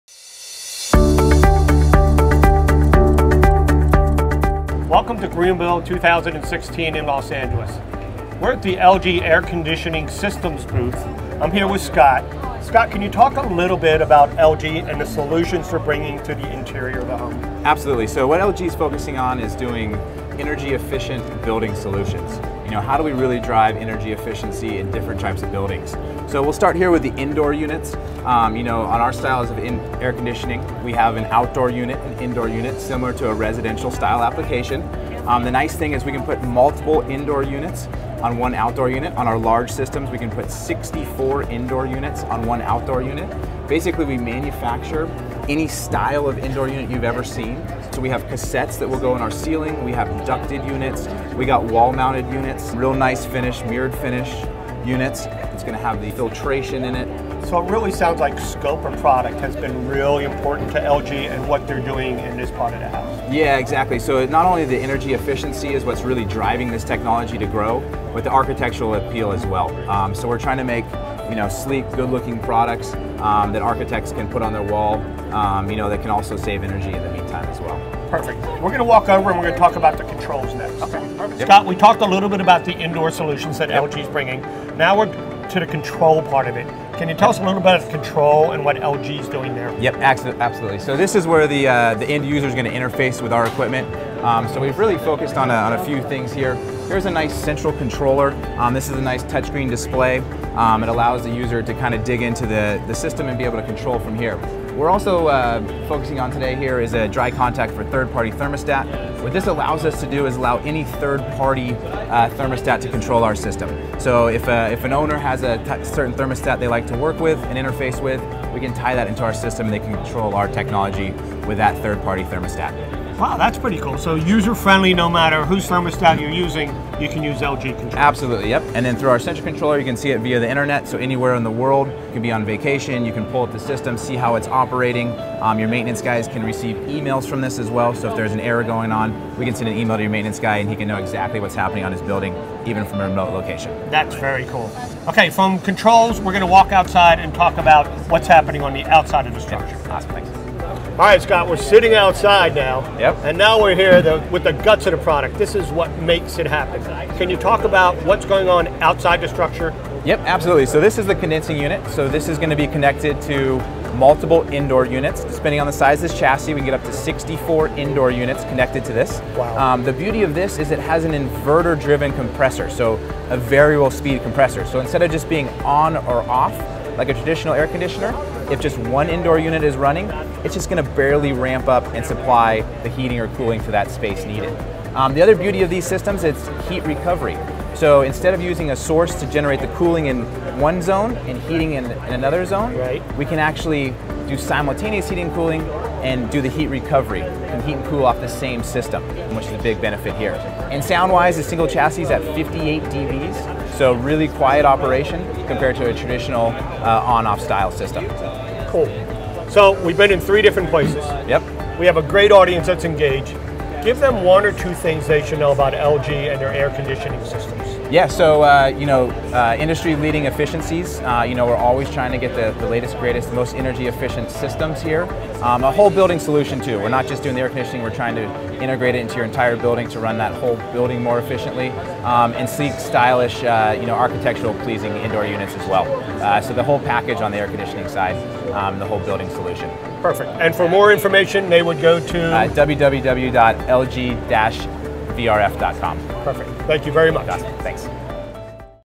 Latest LG HVAC solutions at Greenbuild 2016
LG Regional Sales Engineer, Scott Gilchrist, takes you through the latest HVAC solutions showcased at Greenbuild 2016.